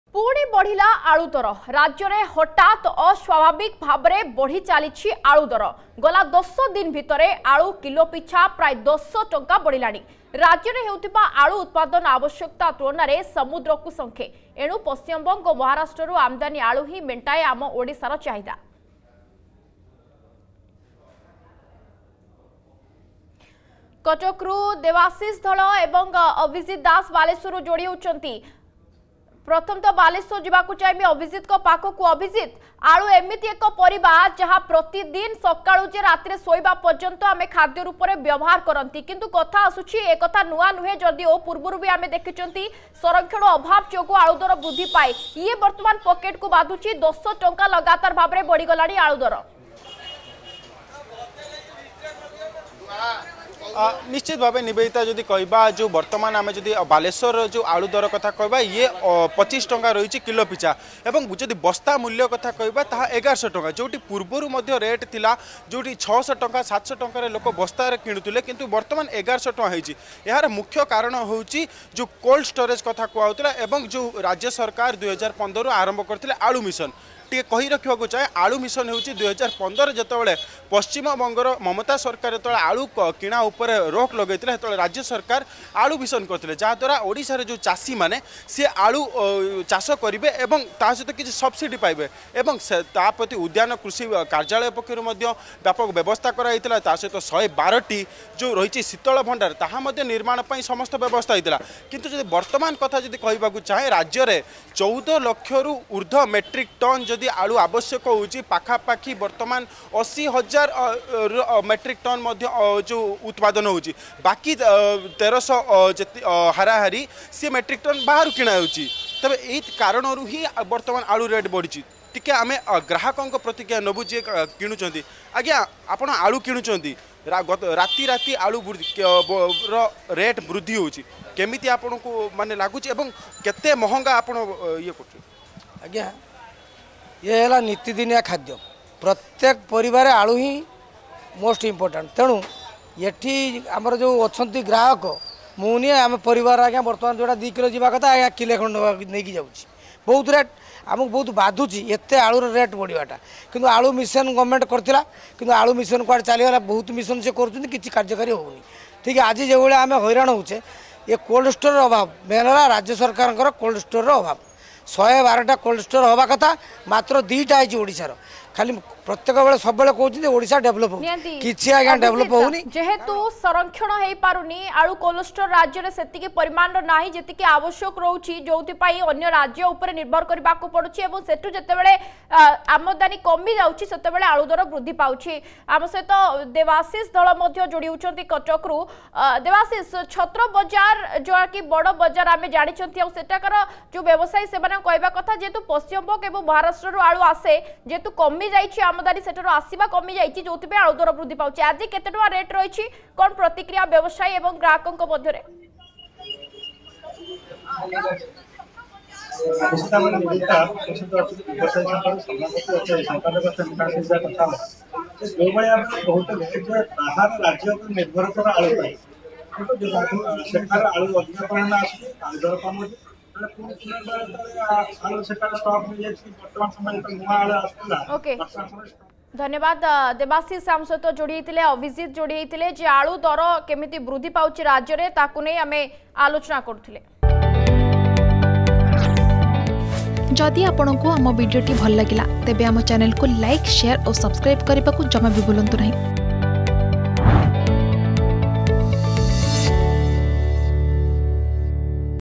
ପୁଣି ବଢିଲା ଆଳୁ ଦର || Potato Prices also Increased
ପୁଣି ବଢିଲା ଆଳୁ ଦର । ରାଜ୍ୟରେ ହଠାତ୍ ଅସ୍ବାଭାବିକ ଭାବେ ବଢ଼ିଚାଲିଛି ଆଳୁ ଦର। ଗଲା ଦଶ ଦିନ ଭିତରେ ଆଳୁ କିଲୋ ପିଛା ପ୍ରାୟ ୧୦ଟଙ୍କା ବଢ଼ିଲାଣି।